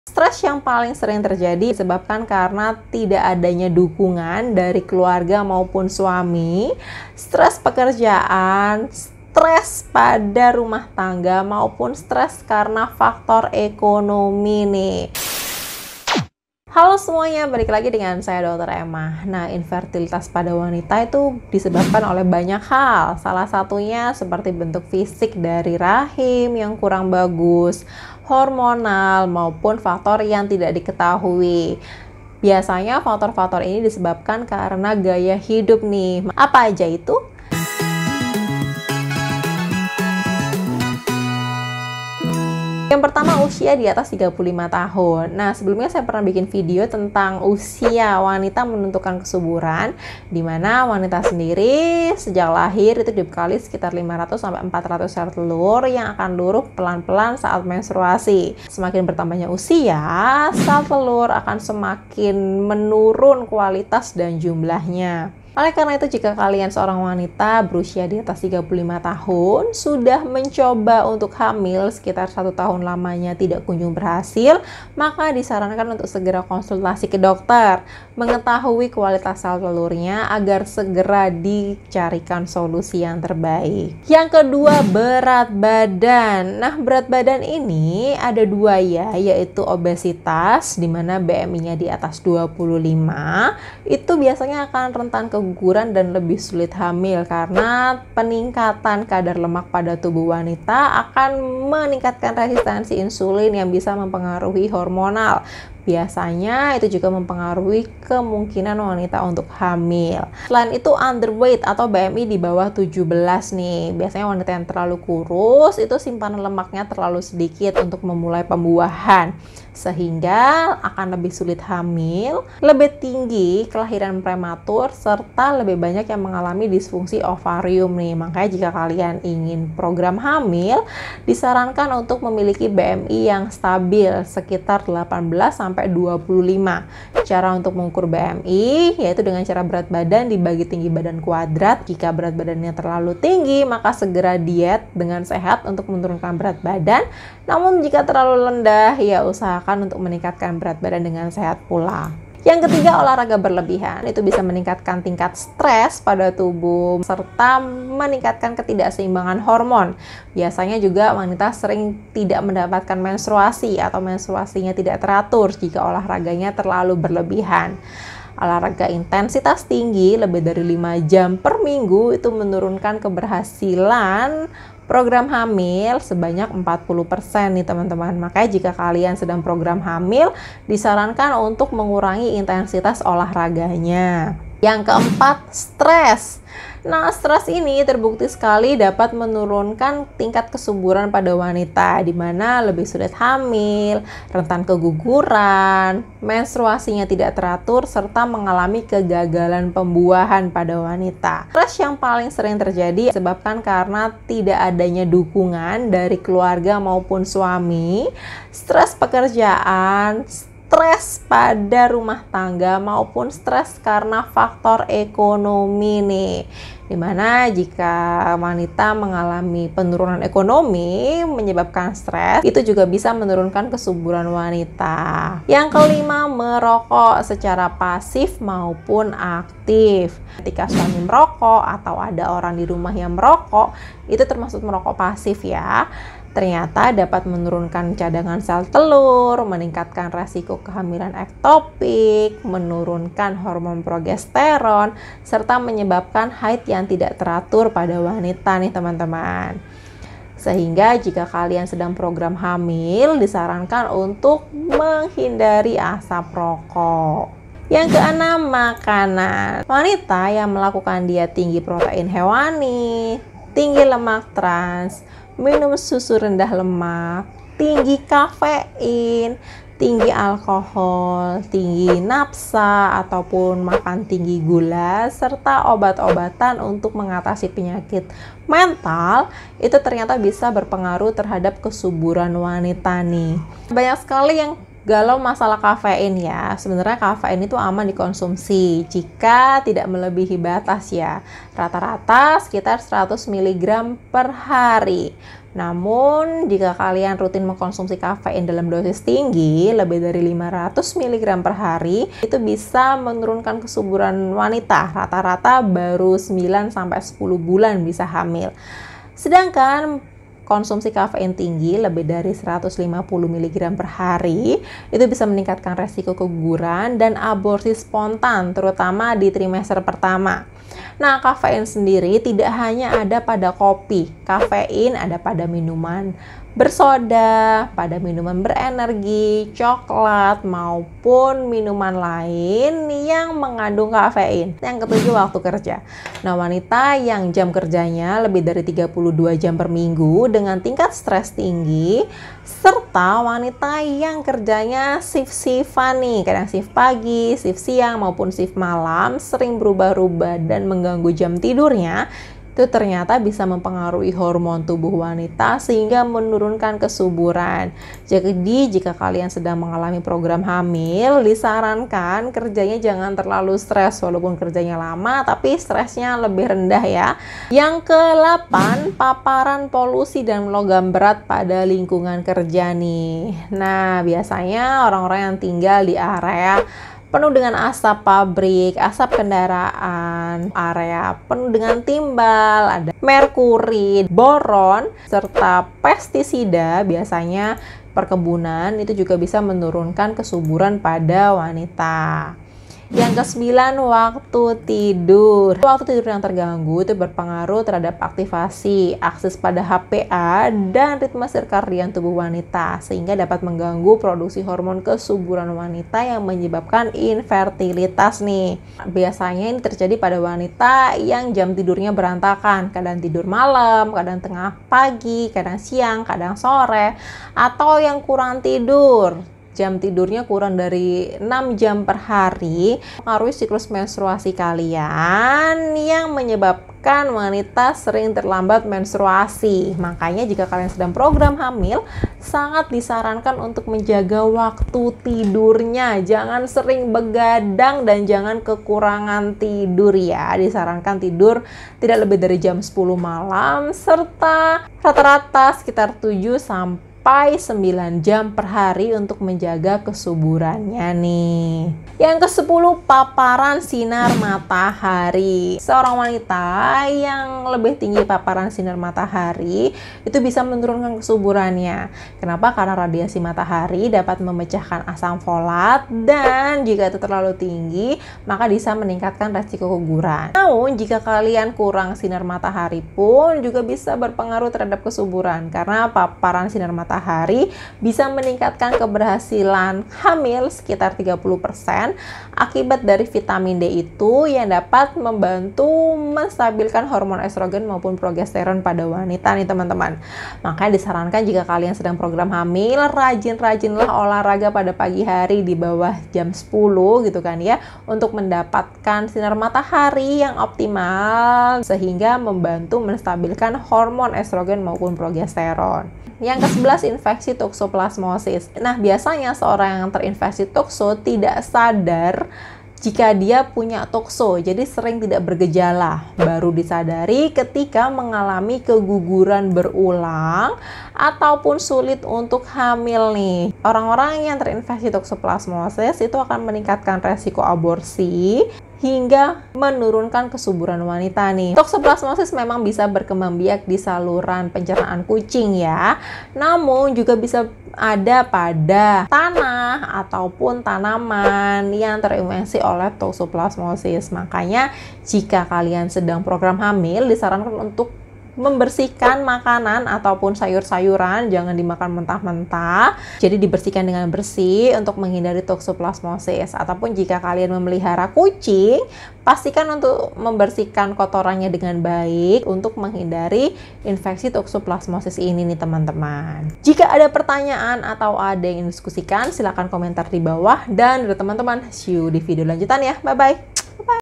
11 KEBIASAAN MENURUNKAN KESUBURAN WANITA, PENYEBAB SULIT HAMIL ! | dr.Emasuperr
Halo semuanya, balik lagi sama aku ema!
Kali ini aku ingin menjelaskan tentang 11 KEBIASAAN WANITA PENYEBAB SULIT HAMIL ! | dr. Emasuperr
11 KEBIASAAN WANITA PENYEBAB SULIT HAMIL !
00:00 Intro
00:42 usia di atas 35 th
01:23 berat badan
02:32 olahraga berlebihan
03:04 stres
03:45 merokok aktif dan pasif
04:16 makanan
05:34 waktu kerja
06:07 paparan polusi dan logam berat
06:31 waktu tidur
07:31 paparan sinar matahari
08:24 infeksi toksoplasmosis

Bantu saya untuk terus membuat konten kesehatan bermanfaat dengan memberikan dukungan melalui . Terimakasih